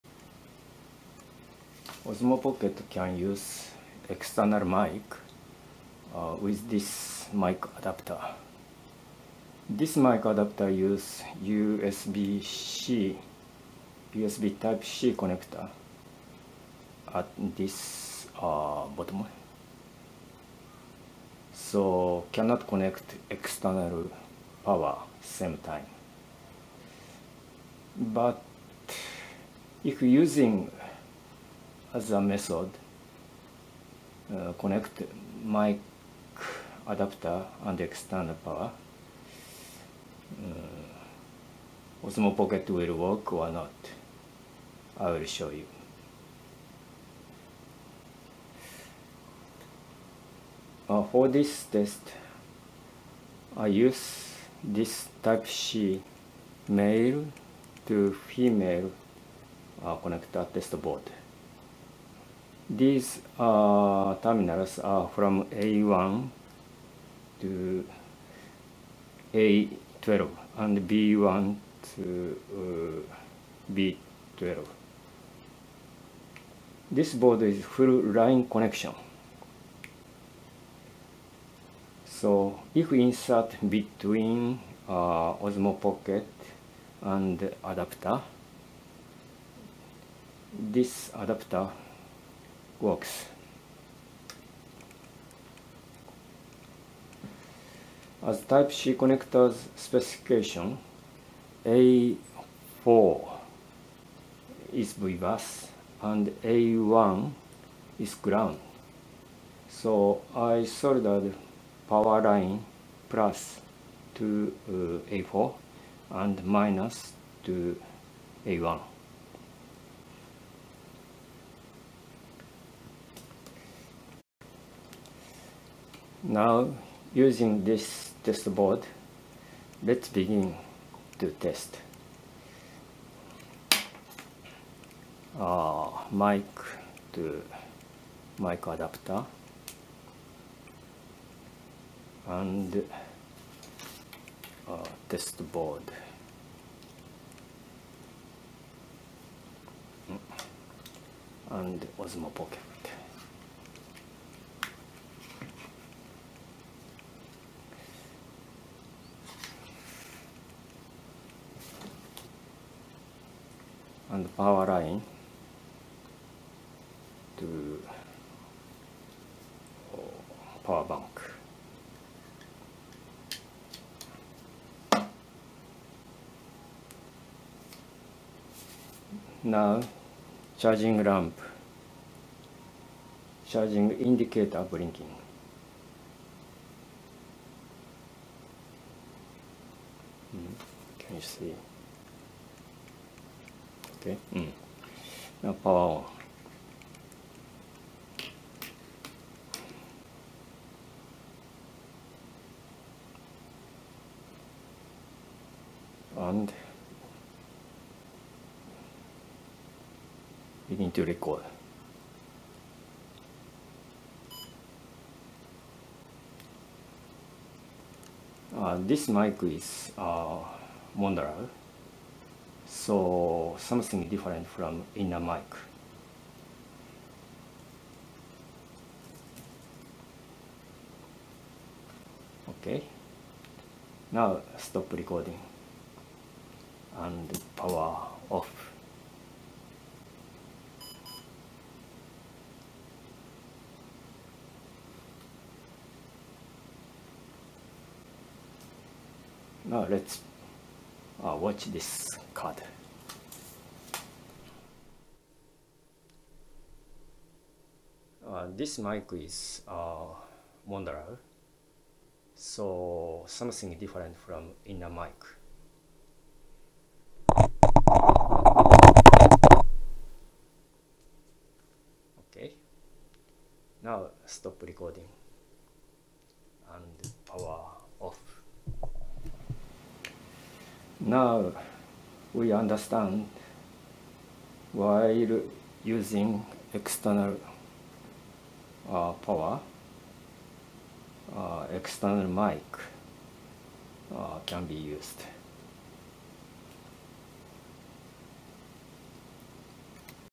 Osmo Pocket Mic Adapter with External Power
By this method, external mic and external power can be used at the same time on Osmo Pocket.